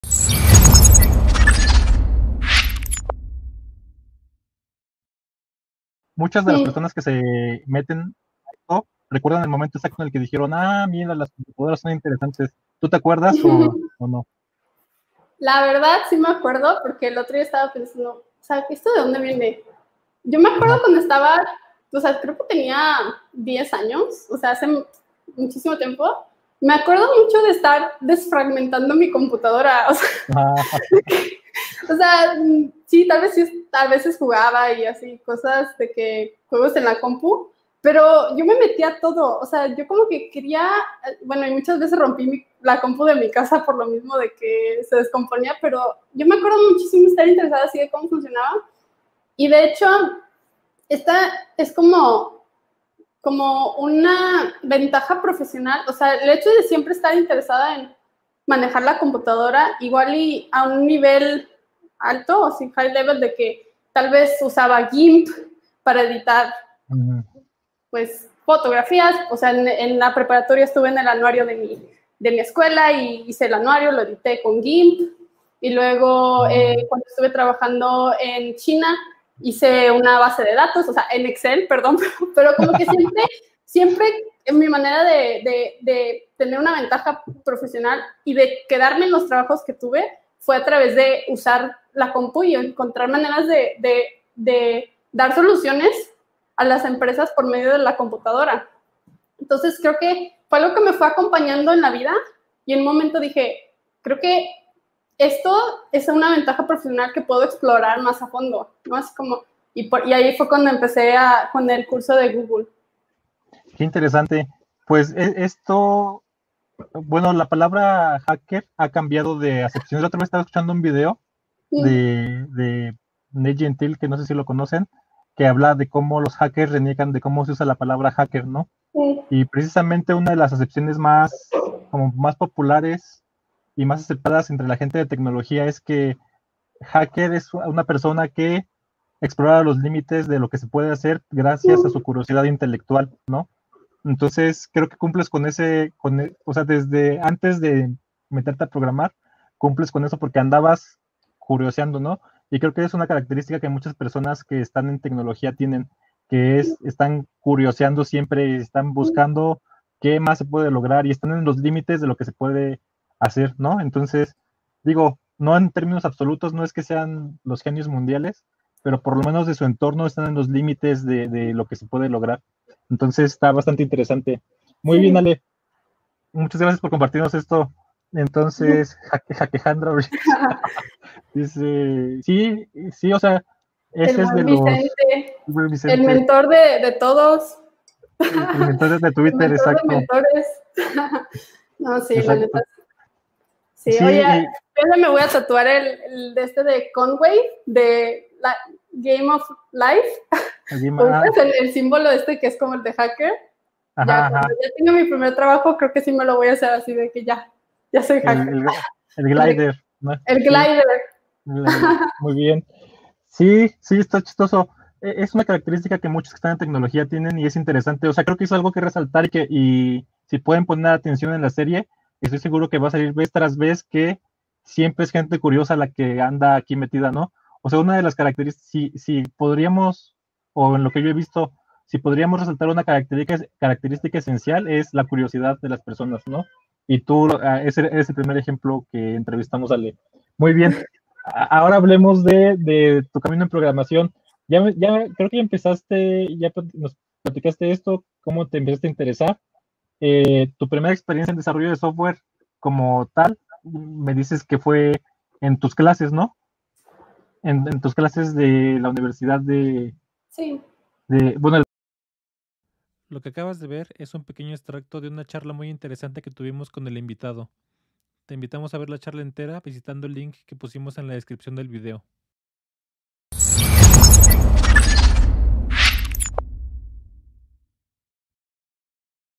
Usar la tecnología como ventaja competitiva | Ale Bricio | Bites de conocimiento
Ale nos cuenta cómo es que siempre uso su conocimiento y curiosidad sobre tecnología como ventajas competitivas en el trabajo y otras áreas de su vida.

Esto es un pequeño fragmento de la entrevista "Cómo me convertí en desarrollador@" que tuvimos con ella.